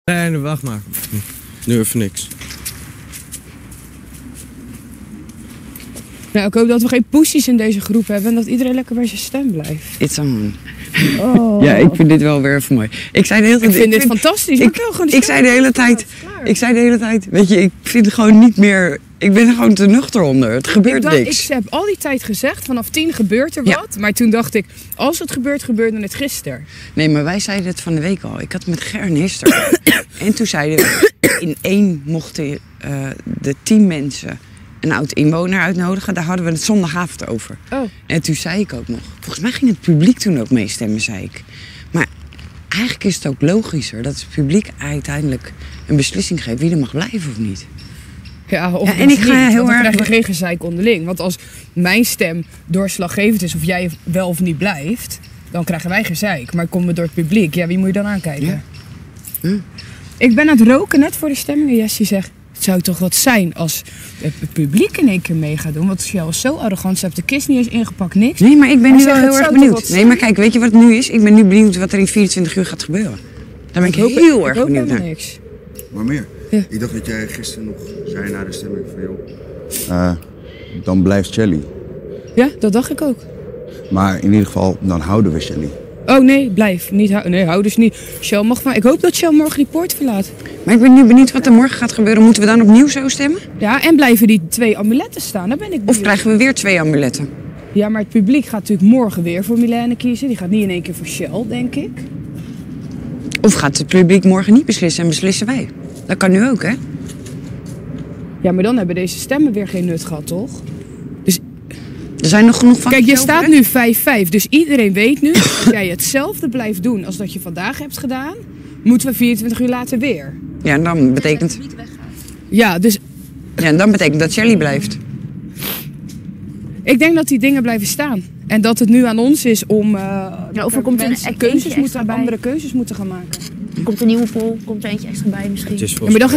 Jessie en merel snappen de positieve stemmen niet
Jessie en Merel woest. Ze snappen de positieve stemmen niet en spreken mensen er op een nare manier op aan.